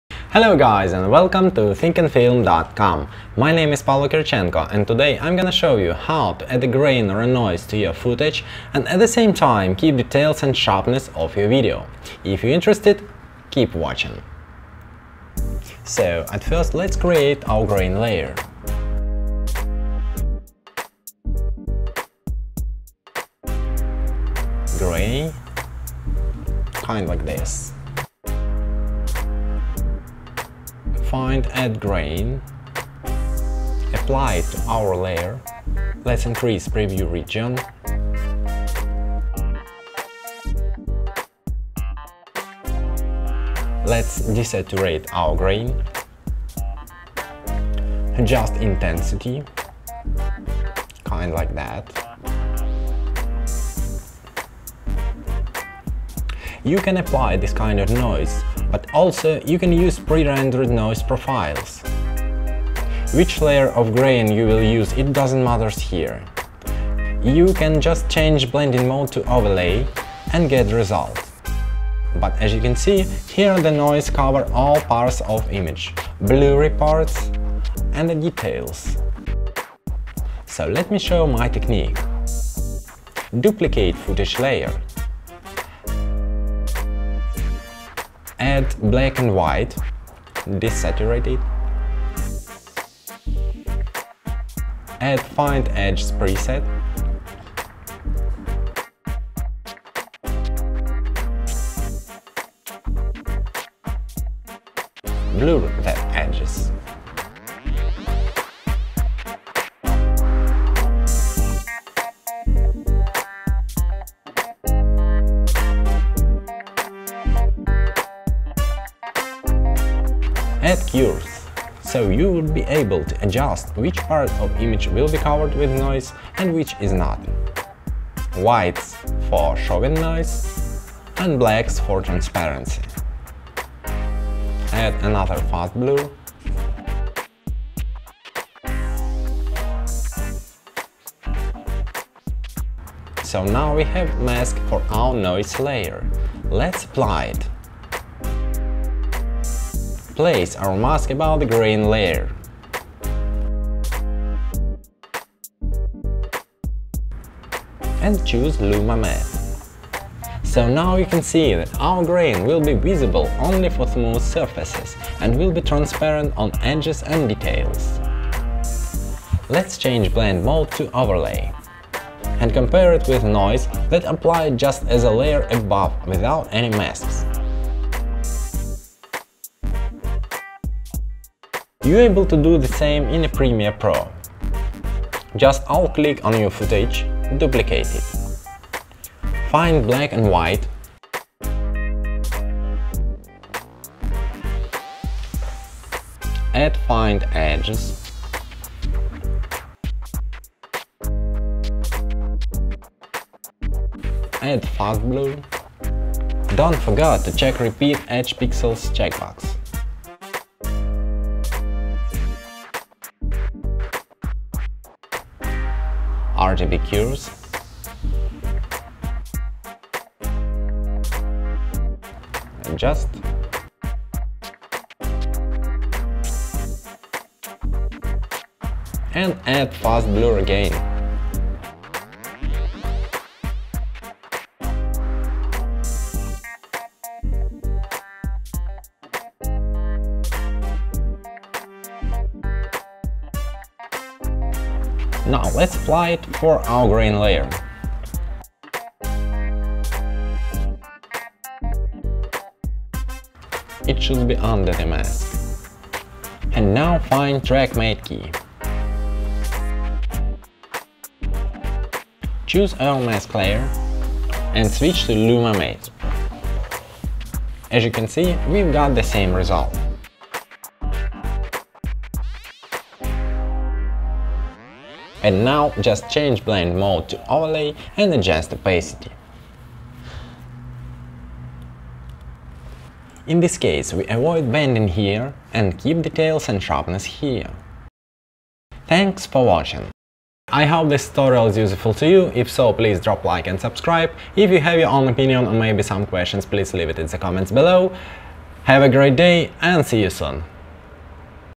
Apply film grain or noise to your footage - advanced technique.
If you going to add some film grain to your footage or remove banding using noise this editing tutorial for you. It explaines, how to apply grain to your video and at the same time do not loose sharpness and detailes.